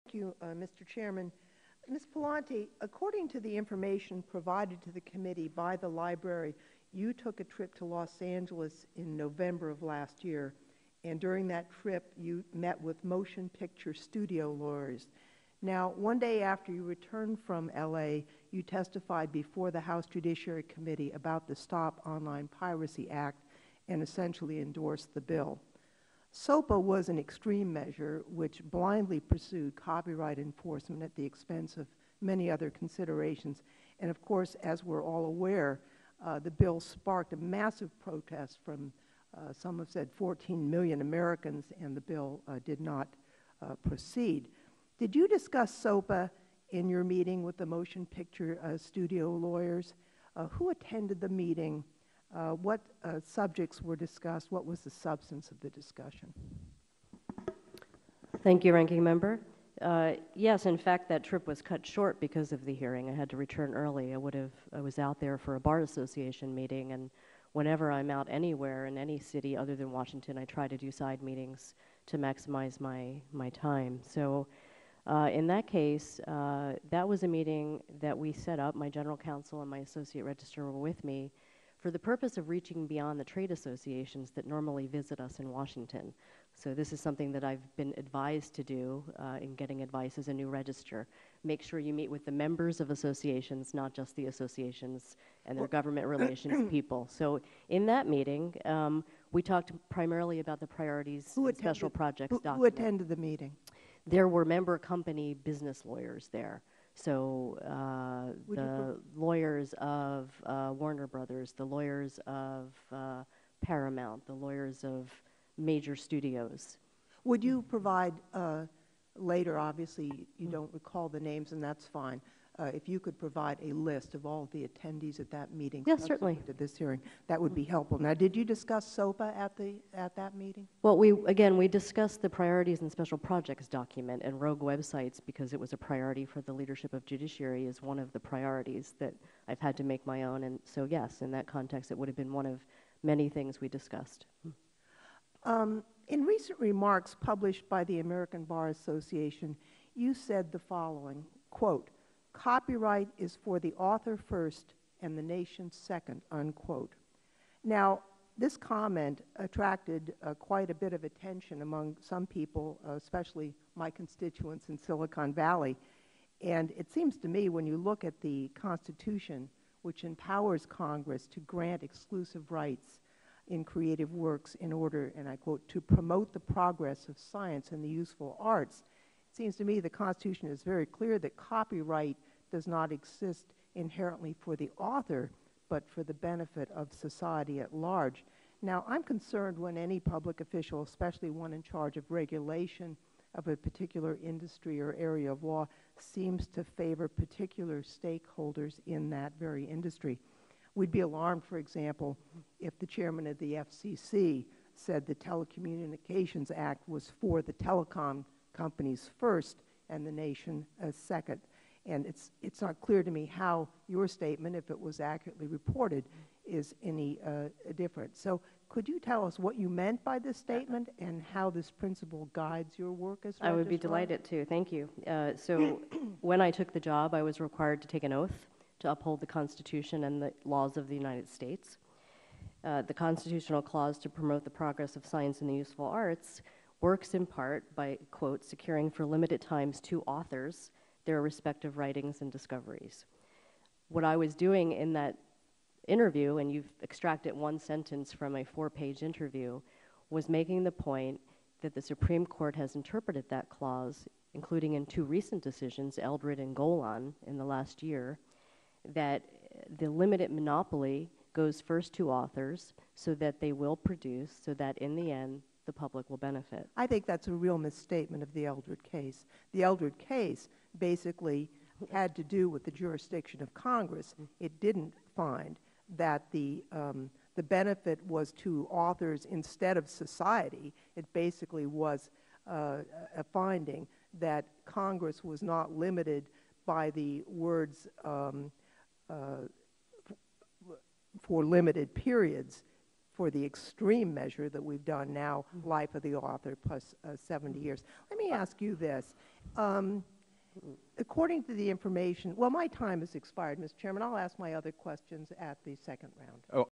Rep. Zoe Lofgren's Q&A Session on SOPA at Library of Congress Oversight Hearing
Rep. Zoe Lofgren's first question and answer session with Ms. Maria A. Pallante, Register of Copyrights for the U.S. Copyright Office, about her November 2011 meetings with motion picture executives in Los Angeles.  After returning to Washington, DC, Ms. Pallante essentially endorsed the controversial SOPA legislation being considered at the time.

Rep. Lofgren is the top Democrat on the Oversight Subcommittee.